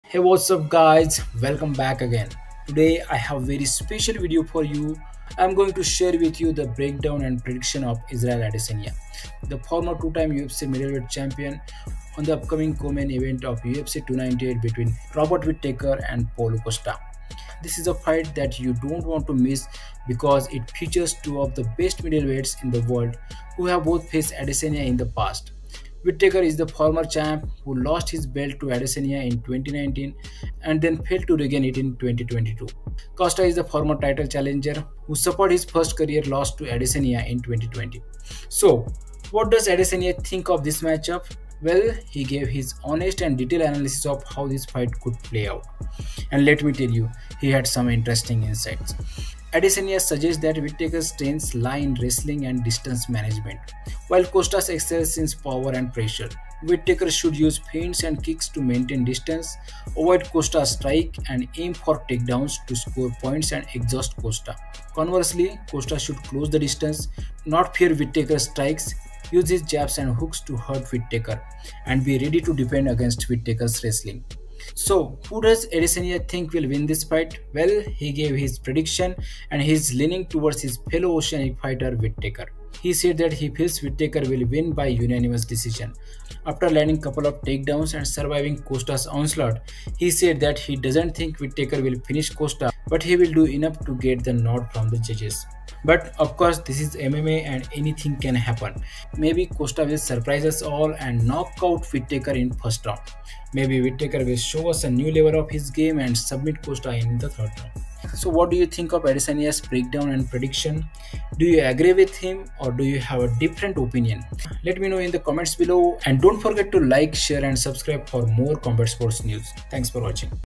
Israel Adesanya Predicts: Robert Whittaker vs. Paulo Costa at UFC 298 | Styleblender
In this must-watch video, former UFC middleweight champ Israel Adesanya dives deep into the upcoming clash between Robert Whittaker and Paulo Costa at UFC 298. Adesanya, having faced both fighters, provides a unique and detailed breakdown of the stylistic matchup, offering insights into Whittaker's wrestling strategy and Costa's pressure game. Don't miss out on Adesanya's predictions and tactical analysis for the co-main event in Anaheim, California!